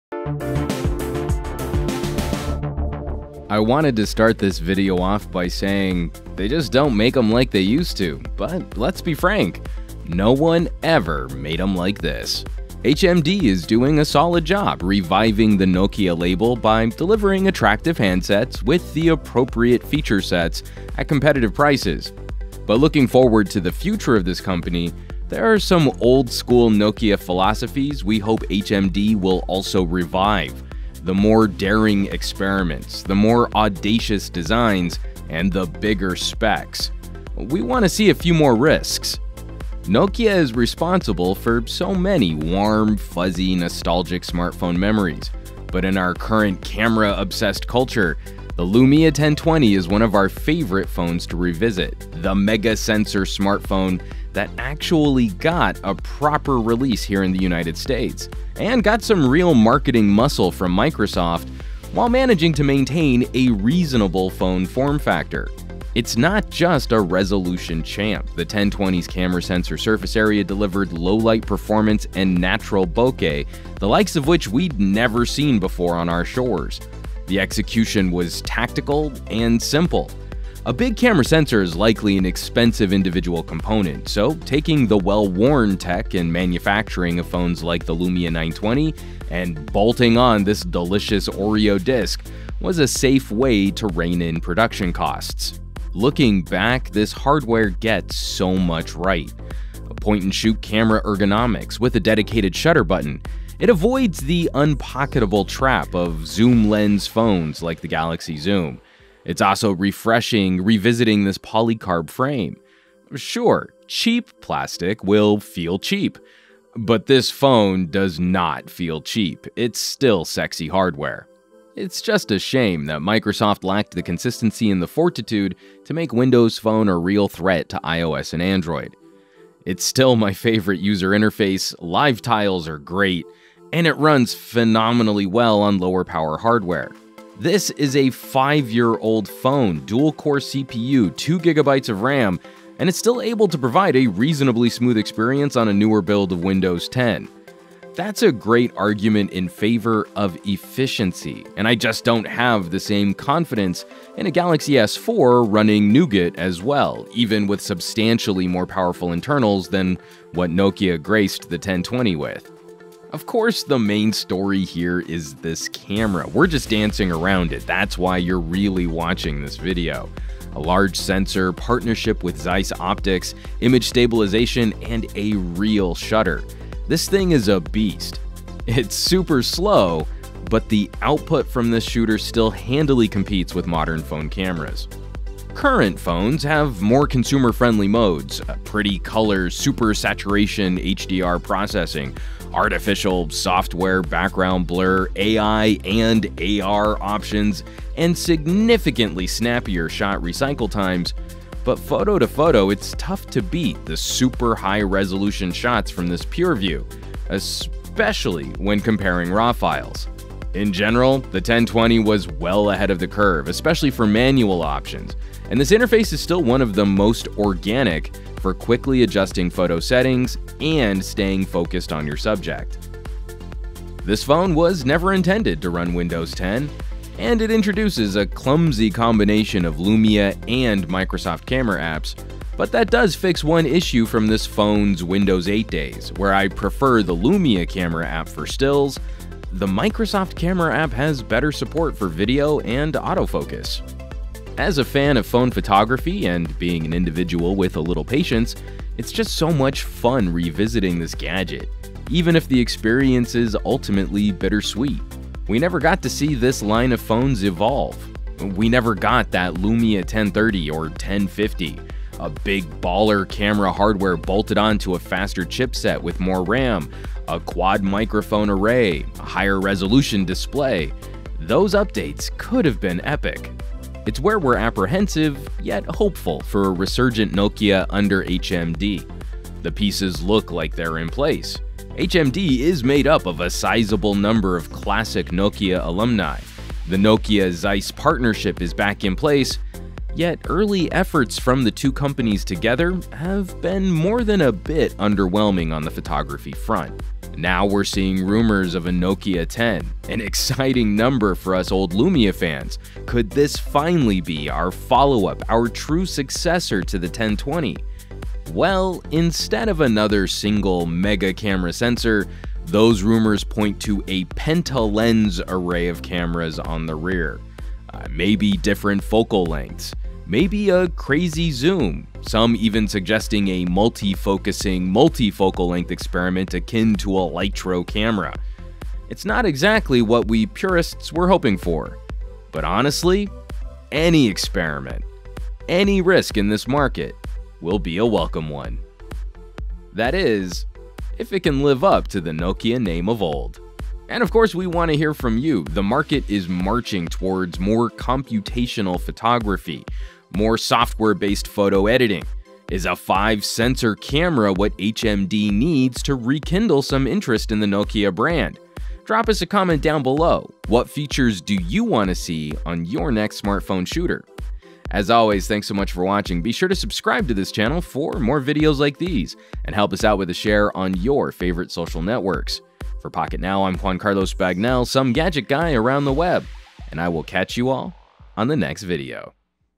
Nokia Lumia 1020 Throwback! Can HMD & Nokia disrupt like this again? | Pocketnow
This is our Nokia Lumia 1020 throwback. HMD is rebuilding the Nokia label with solid first year sales, but what makes a brand really exciting? Looking back to one of the most popular experimental phones of all time, what can HMD learn from the past? Can they push the boundaries on features and hardware? Will we FINALLY get a successor to the Nokia Lumia 1020? How badly would you like that?

Juan's Photo Book